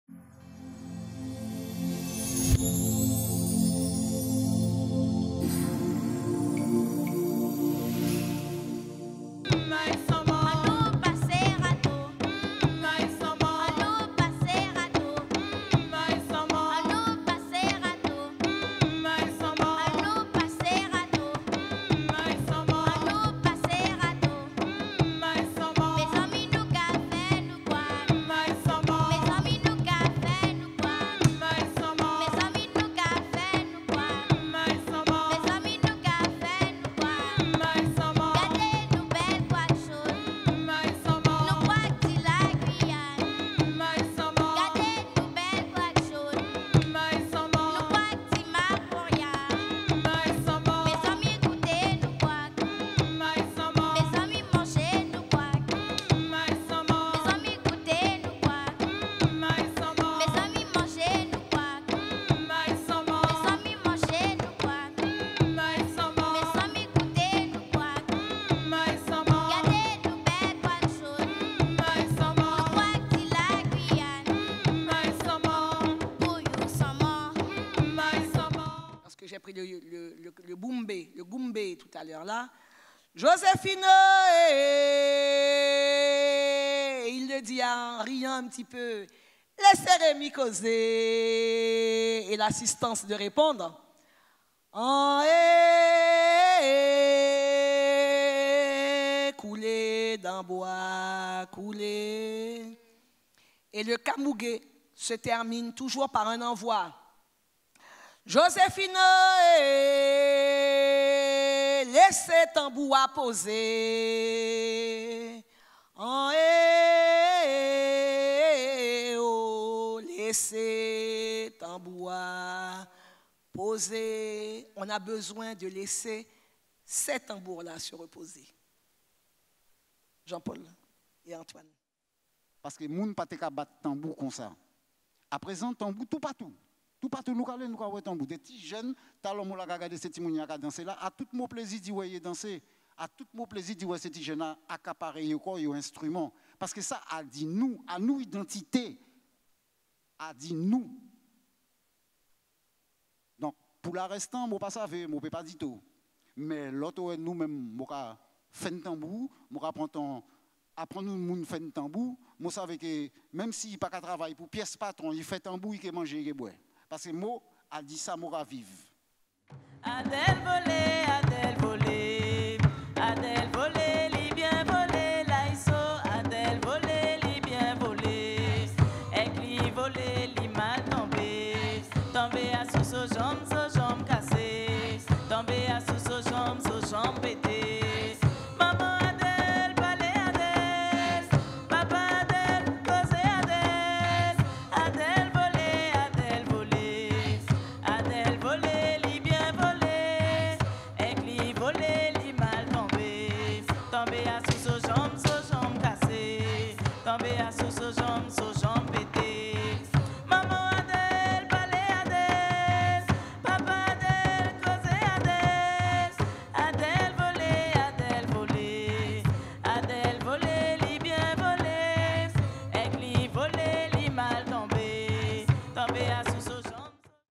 Kamougué Oben Kanmougwé 01 06 2023
La CTG et l'EPCC Les Trois Fleuves accueillaient le 1er Juin 2023 à l'Encre

la Conférence dansée Kamougué oben Kanmougwé
Organisée par le Conservatoire de Musique, Danse et Théâtre de Guyane

Présentation du Kamougué d'hier à aujourd'hui, l'histoire, la danse, l'instrument et les correspondances dans la tradition en Guyane.

Extraits